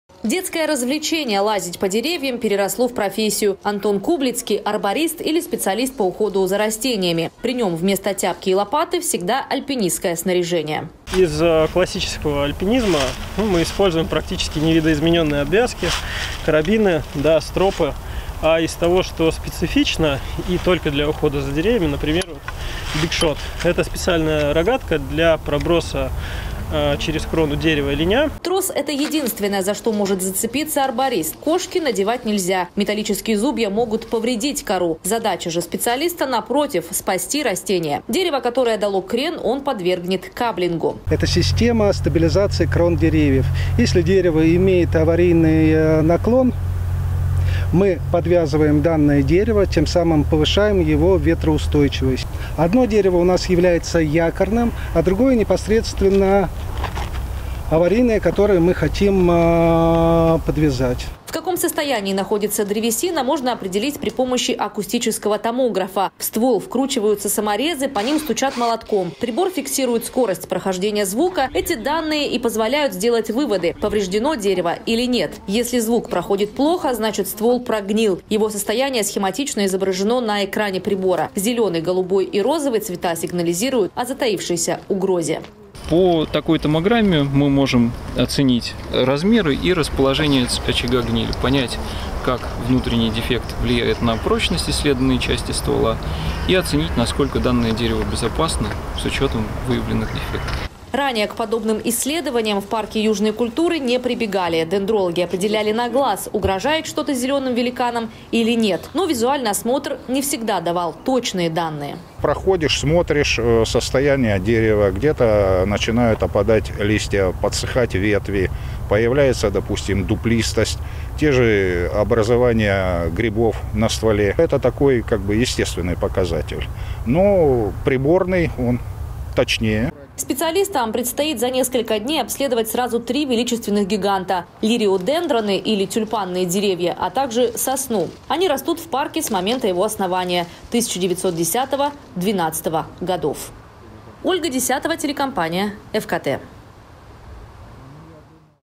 Вековые деревья в Сочи обследуют акустическим томографом. Новости Эфкате
Состояние растений, которые входят в коллекцию парка “Южные культуры”, определяют при помощи акустического томографа. Исследования на протяжении нескольких дней будут проводить московские специалисты. Они же помогут сохранить аварийные деревья, которые дали крен.

В каком состоянии находится древесина, можно определить при помощи акустического томографа. В ствол вкручиваются саморезы, по ним стучат молотком. Прибор фиксирует скорость прохождения звука. Эти данные и позволяют сделать выводы - повреждено дерево или нет. Если звук проходит плохо, значит ствол прогнил. Его состояние схематично изображено на экране прибора. Зелёный, голубой и розовый цвета сигнализируют о затаившейся угрозе.